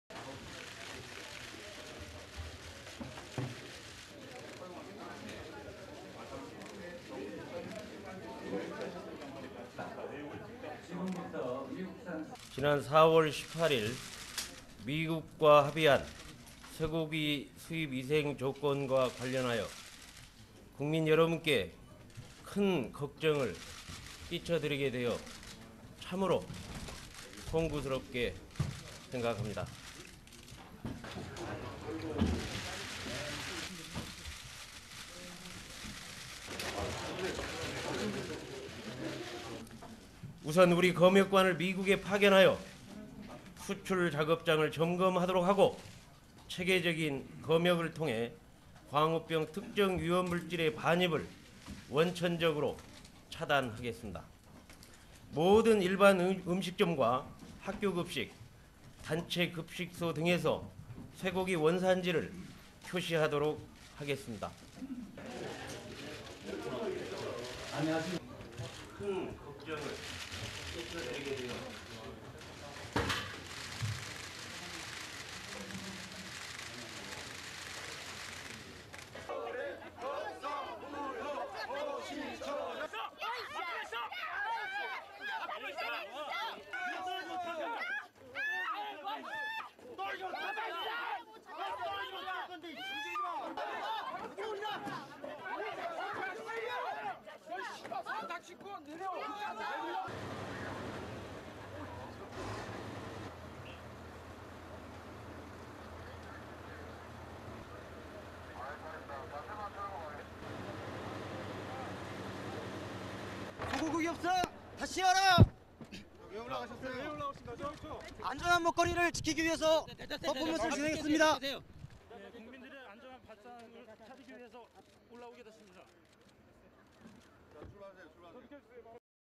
SKorea announces it will implement US beef import agreement
1. South Korean Agriculture Minister Chung Woon-chun walking up to podium
2. Pan from media to Chung
3. Wide of Chung
4. SOUNDBITE (Korean) South Korean Agriculture Minister Chung Woon-chun:
"I feel very sorry that the April 18 agreement of US beef imports has given much concern to the people of the nation."
5. Chung bowing to the media
6. Cutaway of photographers
7. SOUNDBITE (Korean) South Korean Agriculture Minster Chung Woon-chun:
"We'll dispatch our inspectors to the quarantine centres in the US and monitor there, and through the systematic quarantine process, we'll stop any imports containing tainted beef products."
8. Agriculture Ministry officials
9. Chung behind podium
10. Zoom-out of Chung and officials bowing
11. Protesters marching on the streets in front of the ministry
12. Protesters being blocked entry to the ministry by riot police
13. A female protester attempting to climb the fence and being blocked by police
14. Protesters scuffling with protesters
15. Wide shot of two male protesters sitting on top of a McDonald's fast-food chain sign
16. Tight pan of the protesters
17. Mid of a firefighter approaching protesters sitting on sign
18. Mid of firefighters looking up at protesters
19. SOUNDBITE: (Korean) Park Yong-sung, a protester:
"Renegotiate the beef agreement!"
(reporter asking why he climbed on the McDonald's sign)
"I did this to protect the safety of our food."
20. Another protester being put into a police car

STORYLINE:

South Korea's government announced on Thursday that it is going forward with a much-criticised deal to resume U.S. beef imports, a move that could intensify continuing street protests against the government over fears of mad cow disease.

Agriculture Minister Chung Woon-chun said in a nationally televised announcement that the government is easing quarantine regulations on US beef in accordance with the April 18 agreement with Washington.

However, Chung said the government would still monitor the situation closely.

"We'll dispatch our inspectors to the quarantine centres in the US and monitor there, and through the systematic quarantine process, we'll stop any imports containing tainted beef products," he said.

The relaxed rules will take effect as soon as they are published in a government journal in a few days.

Thursday's announcement, which had been delayed in recent weeks amid anti-government protests, was the final administrative step necessary to resume US beef imports.

It cleared the way for American beef to return to South Korean store shelves for the first time since last year when limited imports were briefly allowed before again being suspended.

The move is likely to intensify anti-government rallies in Seoul, which have been held on a near-daily basis in recent weeks to protest against the beef agreement.

Protesters believe the accord does not adequately protect the country from infected beef.

Park Yong-sung, a 37-year old protester called for a renegotiation of the agreement.

Park and another demonstrator were removed by firefighters from a McDonald's fast-food chain sign, where they were sitting on a sign above the restaurant in protest.

He said he was protesting "to protect the safety of our food."

The deal scrapped nearly all the quarantine restrictions imposed by the previous government to guard against mad cow disease.

South Korea banned U.S. beef after the first case of mad cow disease in the United States was discovered in late 2003.